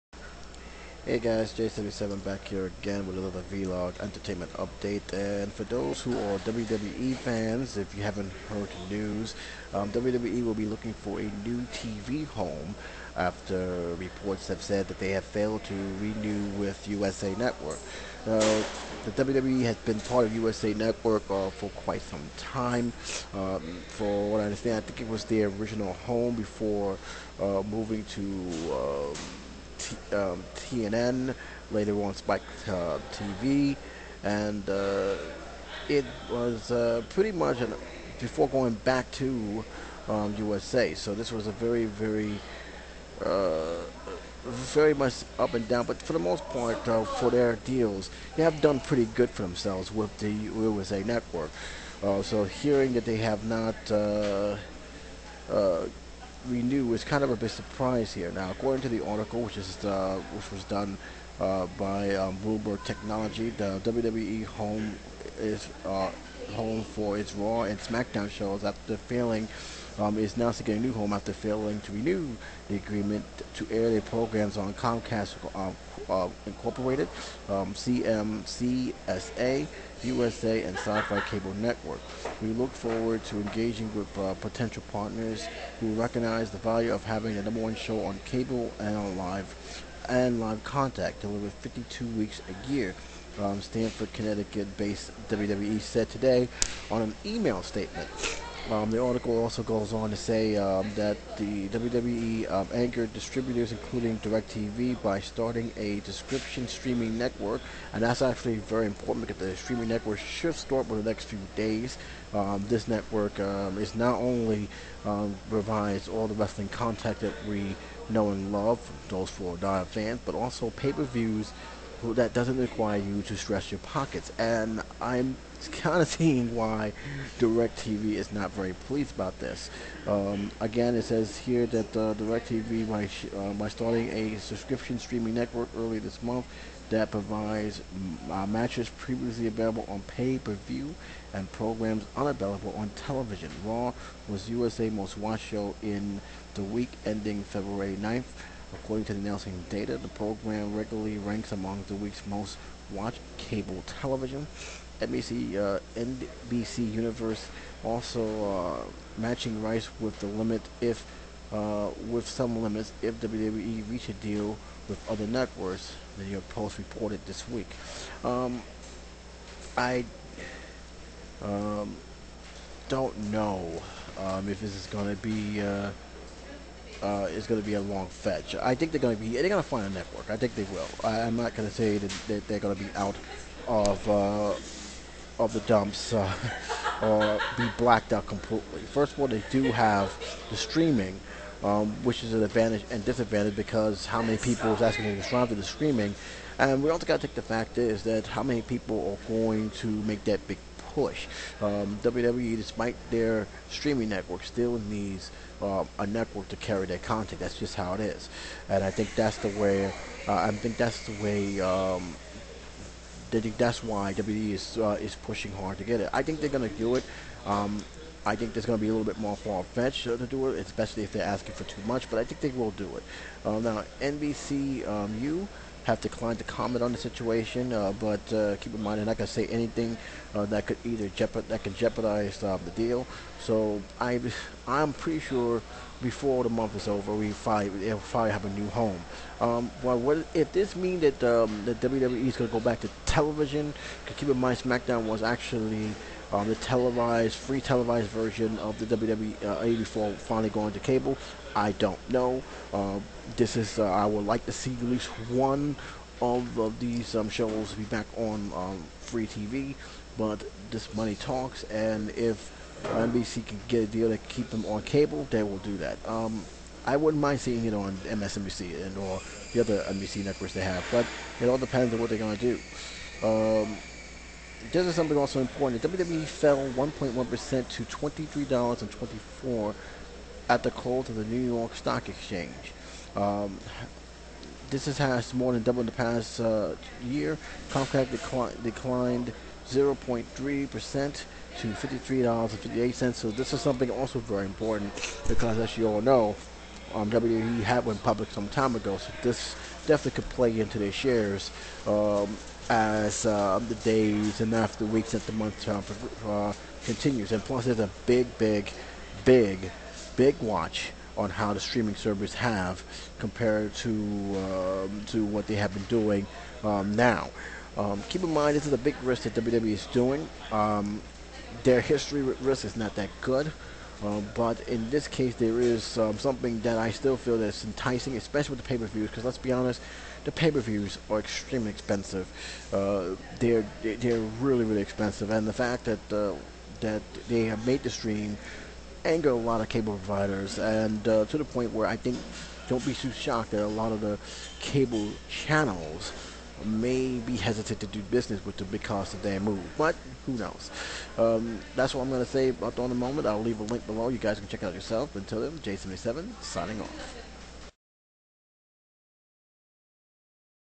WWE Looks For A New TV Home after Losing USA TV Deal
WWE will be looking a new place to post the Raw and SmackDown  tv show after it was confirm it will not returning to the USA Network. NBC Universal  be in the upcoming future? J 77 will give his thoughts on the matter.